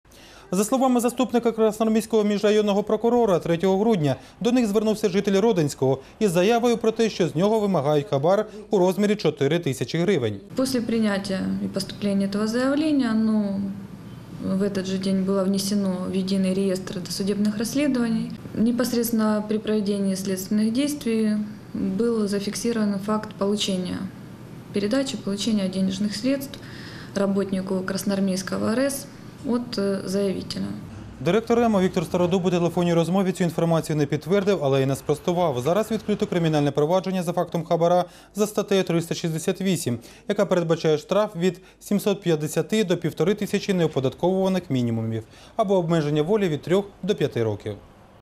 В Красноармейске сотрудников РЭС задержали на взятке
Трех сотрудников Красноармейского РЭС представители правоохранительных органов задержали на взятке.